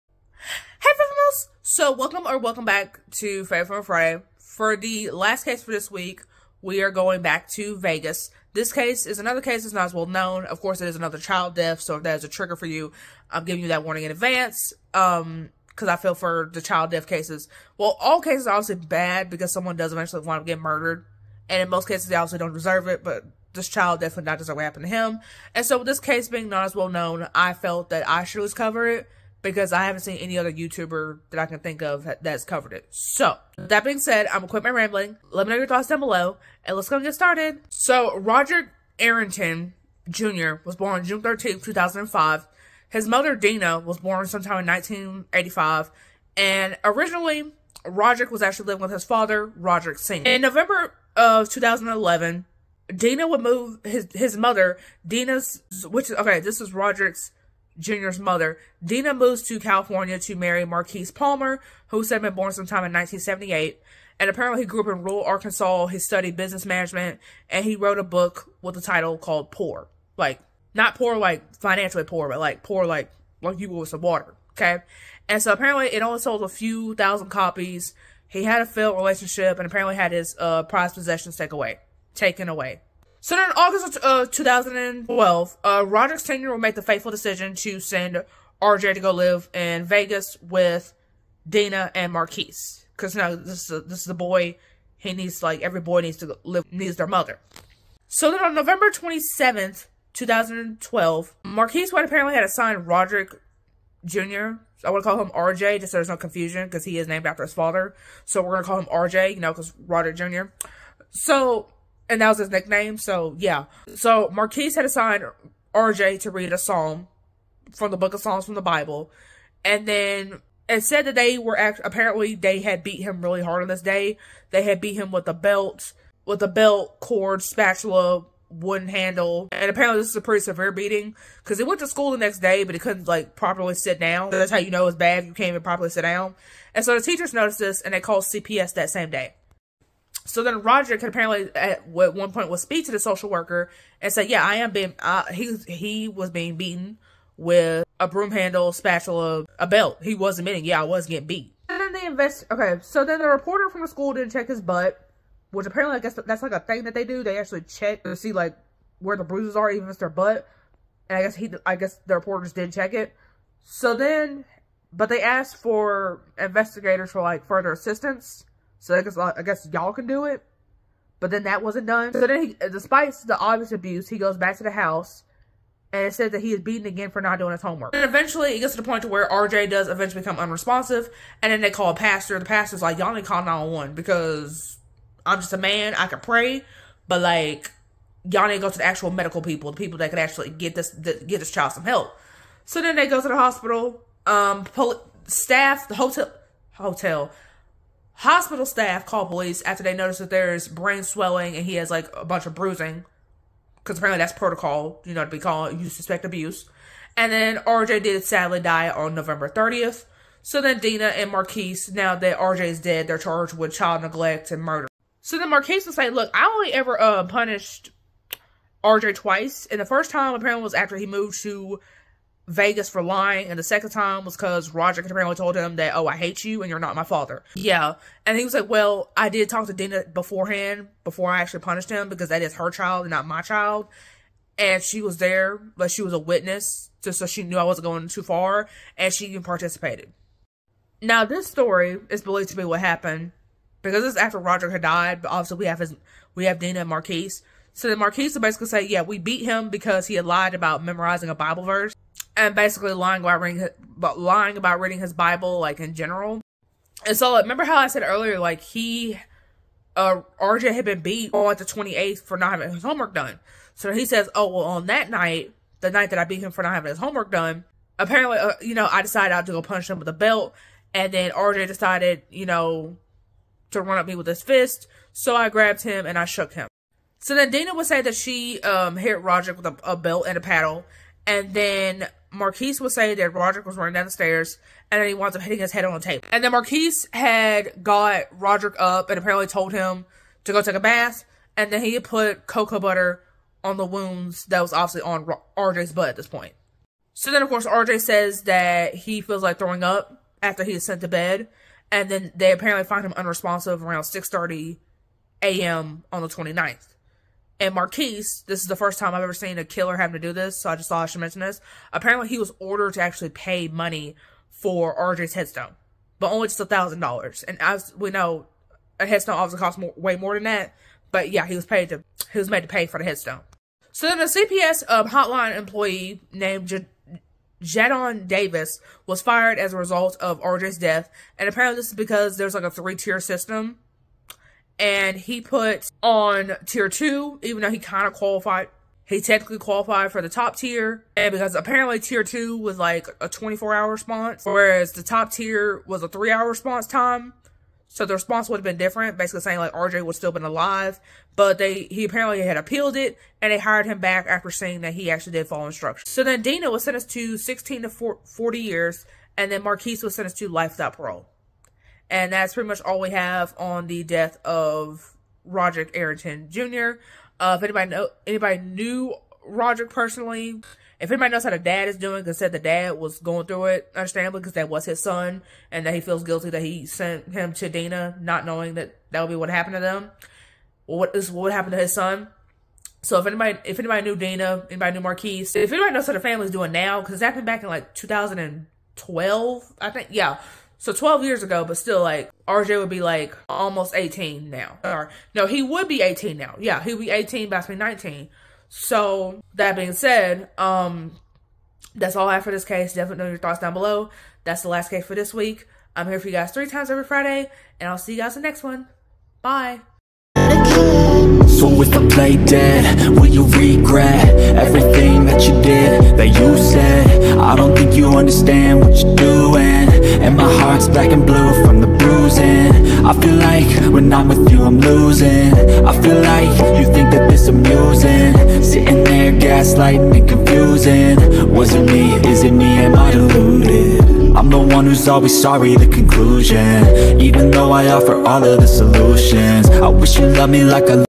Beat over a BIBLE VERSE?! The Murder of Roderick Jr.
Hey Fatal Females! 👋🏾 👋🏾 👋🏾

In 2012, a boy would named Roderick Arrington would go to Vegas to live with his mother & her boyfriend. One night, it's said that he didn't memorize a Bible verse. Within 48 hours, Roderick Jr would be dead & his mother was to blame.

Sources 💫 💫 💫